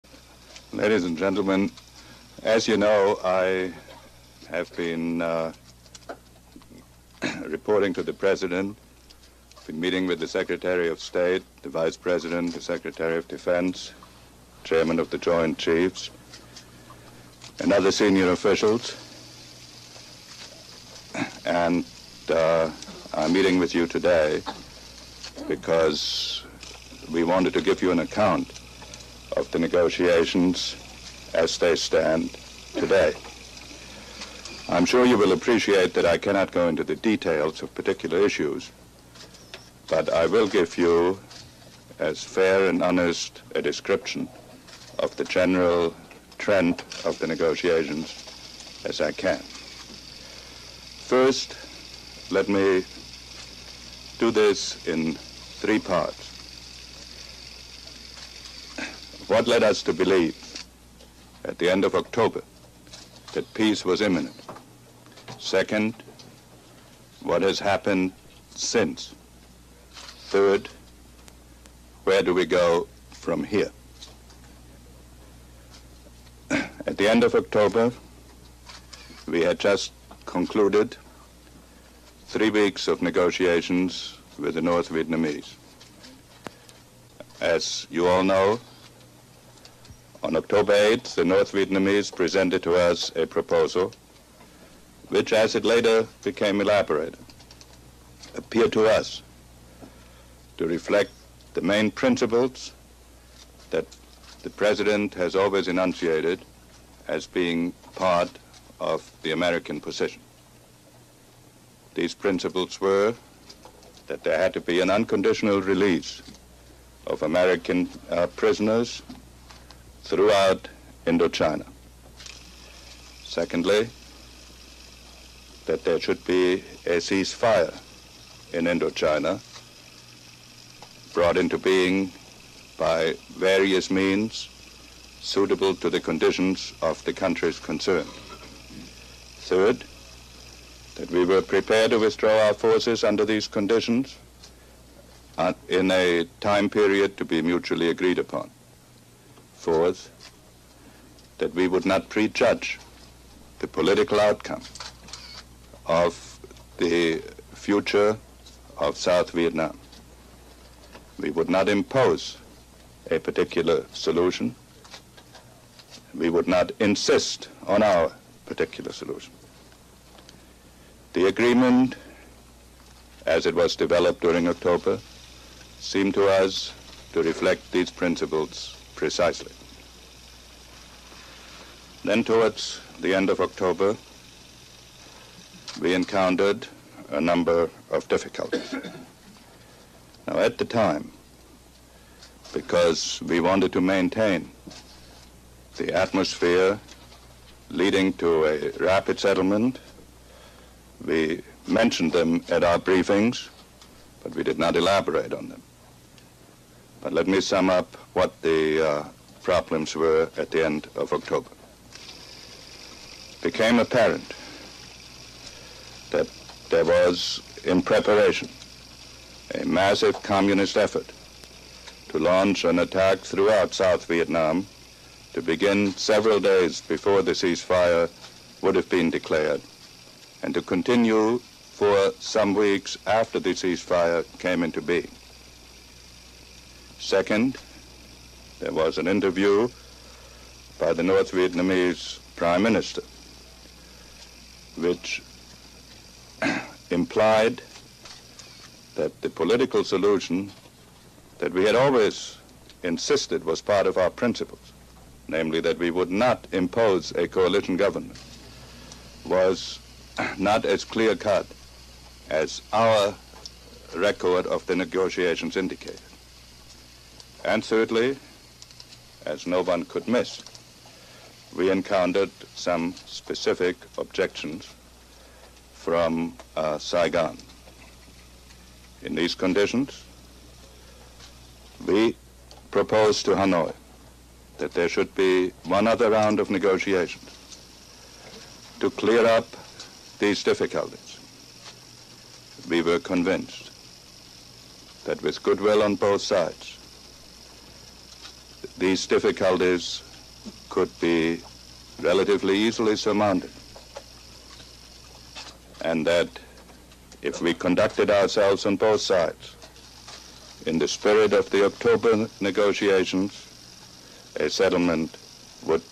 Henry Kissinger's Press Conference on Vietnam Neogotions (December 1972)
Wanna help us create more content?

$onesmallstephistory

Source:
Richard Nixon Presidential Library (NARA)